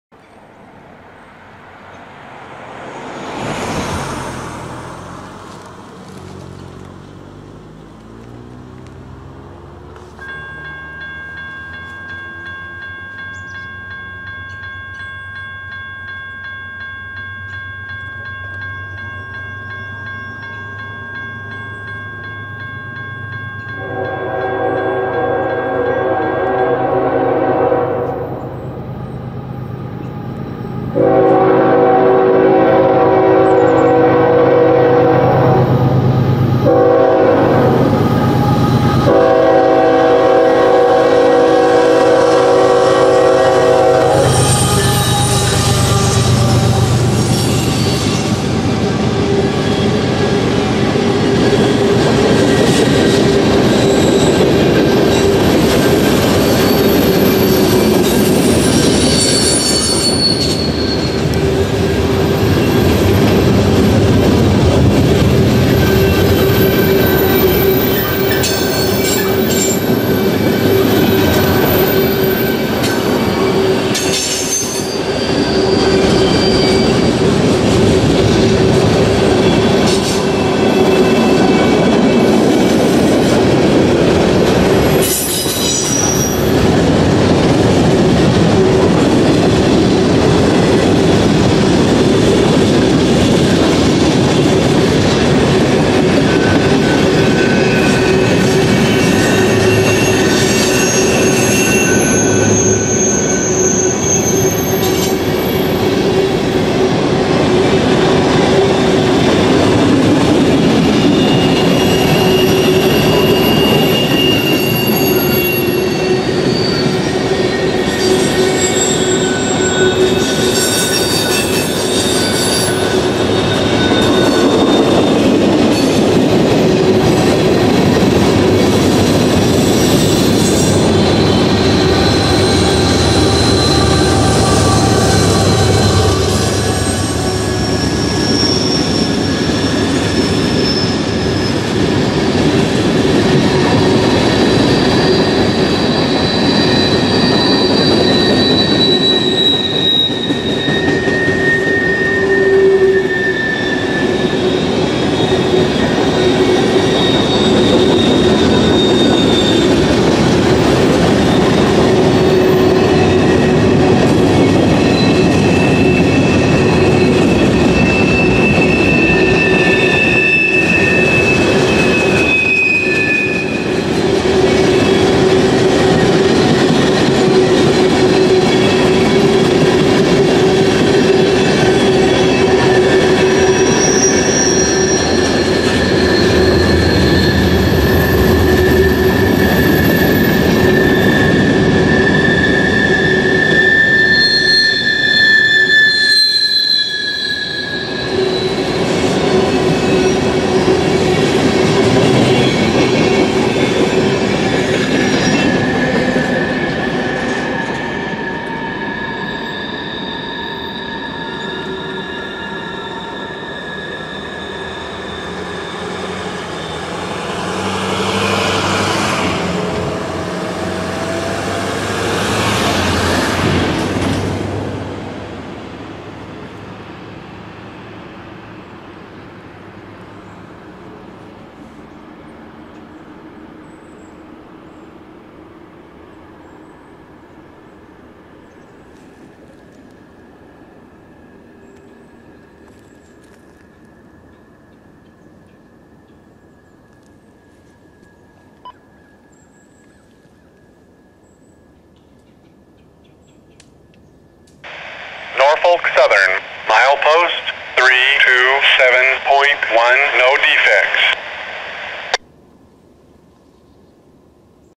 Lower Mill Road Railroad Crossing, Hixson, TN with Defect Detector
Another GS Type 1 and another defect detector.
This crossing is located next to Hixson Defect Detector and the intermediate signal for the siding at Cave Springs.
The crossing has standard NS equipment with Safetran gates, lights, and RECO gate lights.
The defect detector is a Southern Technologies Corp SmartSCAN NG.